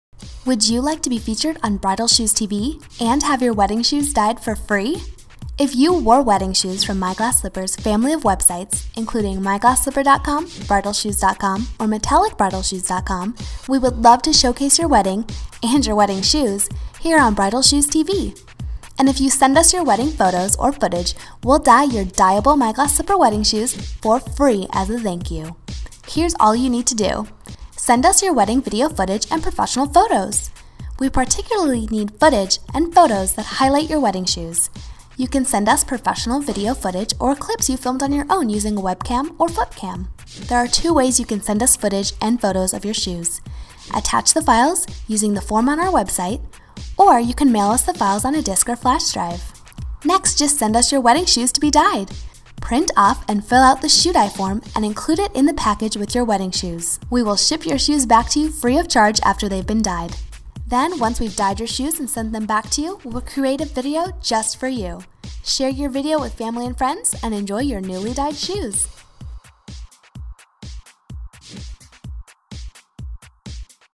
Introduction to Real Weddings Brides Videos on BridalShoesTV
Would you like to be featured on BridalShoesTV and get your wedding shoes dyed for free? If you wore wedding shoes from My Glass Slipper's family of websites (including MyGlassSlipper.com, BridalShoes.com or MetallicBridalShoes.com), we would love to showcase your wedding -- and your wedding shoes -- here on BridalShoesTV!

And if you send us your photos or footage, we'll dye your dyeable My Glass Slipper wedding shoes for FREE!

and click on the pink "Send Us Your Wedding Video" button in the footer. You'll find all the info you need there!